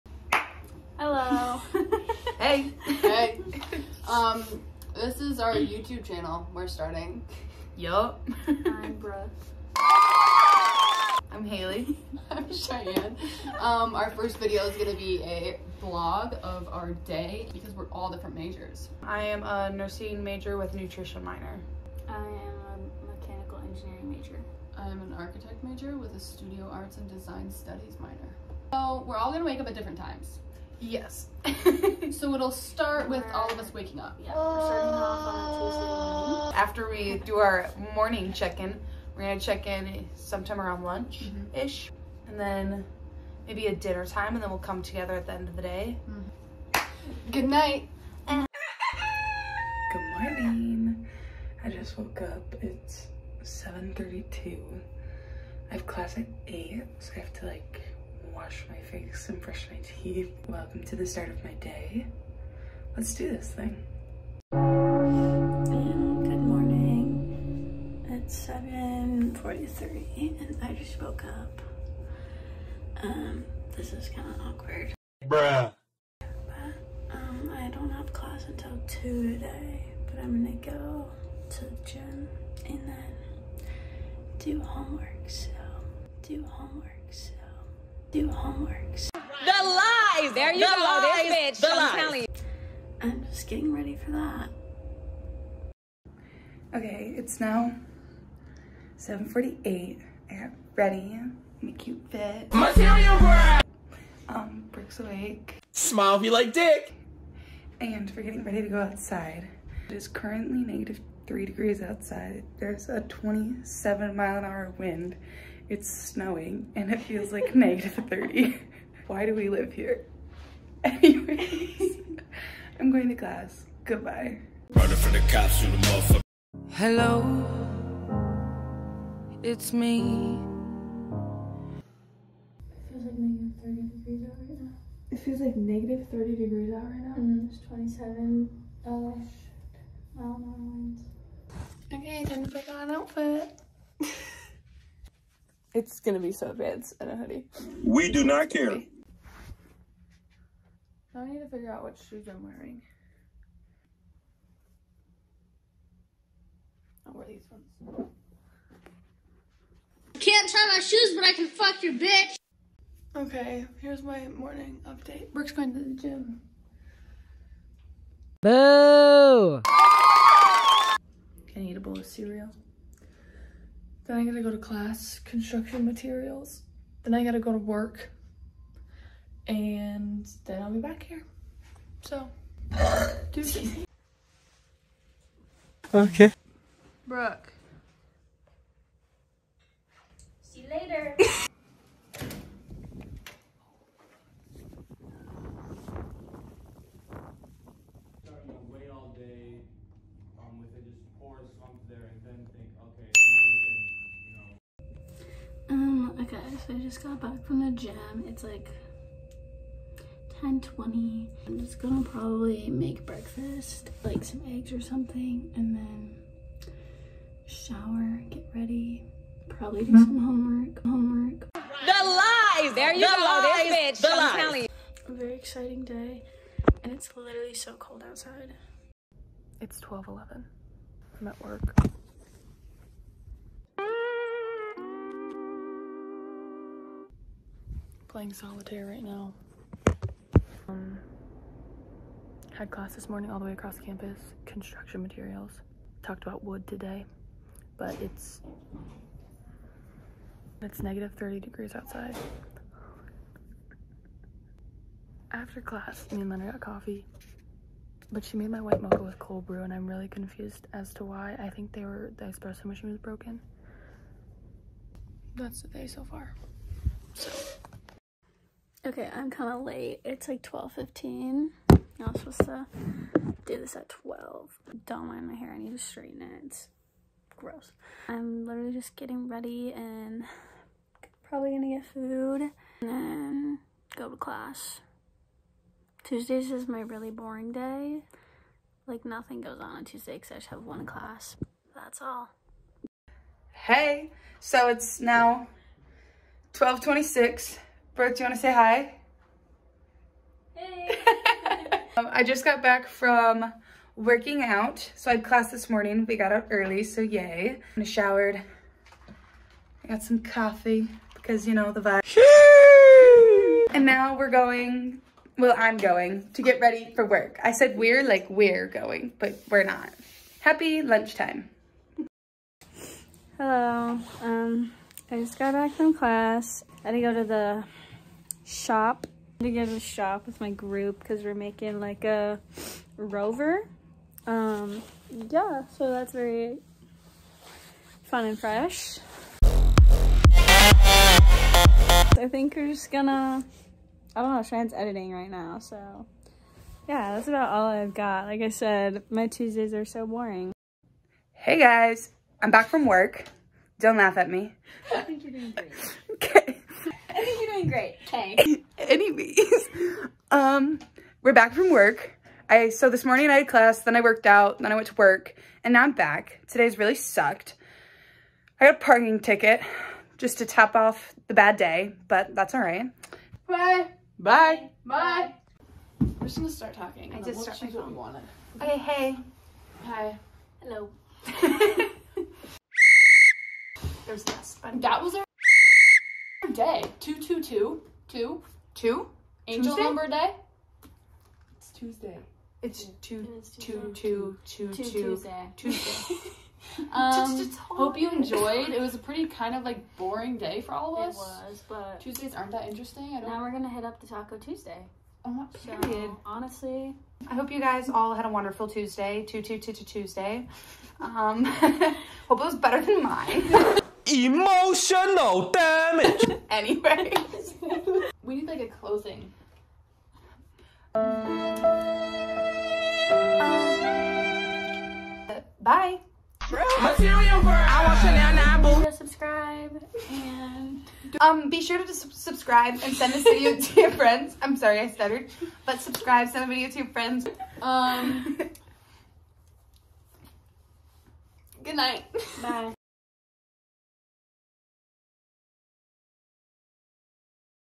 A DAY AS A COLLEGE STUDENT AT SDSU- vlog
Join us for our 2/22/22 Tuesday at South Dakota State University.